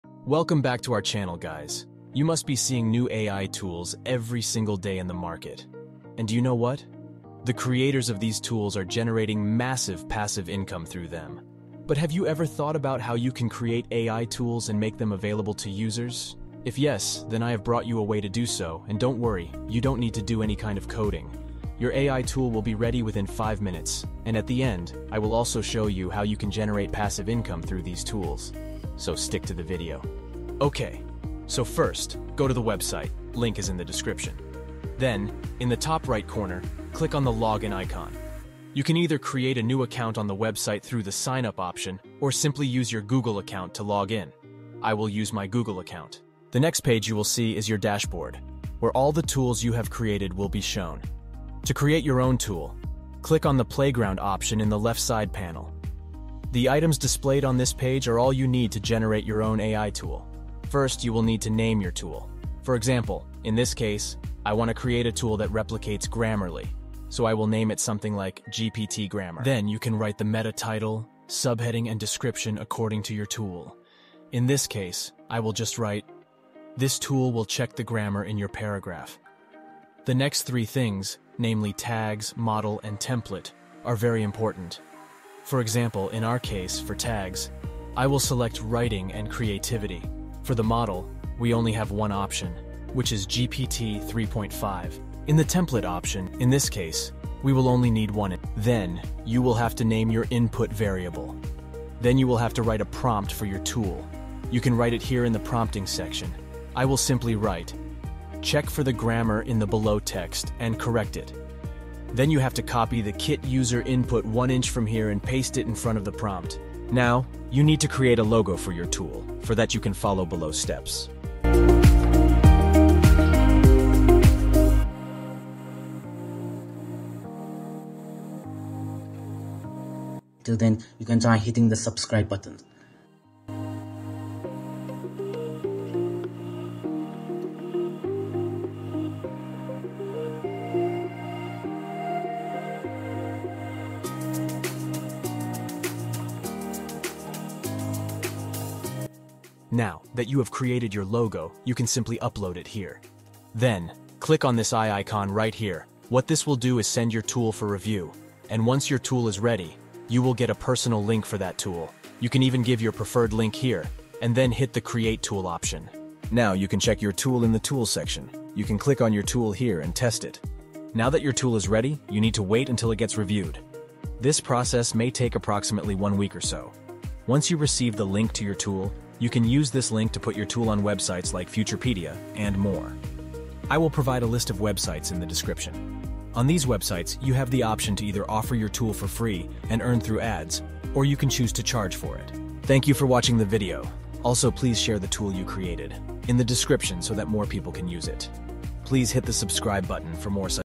Create AI Tools For Free | Sell Your AI tools [Must Watch]
In this video, i have talked about a very easy method to create AI tools free without coding and earn from it.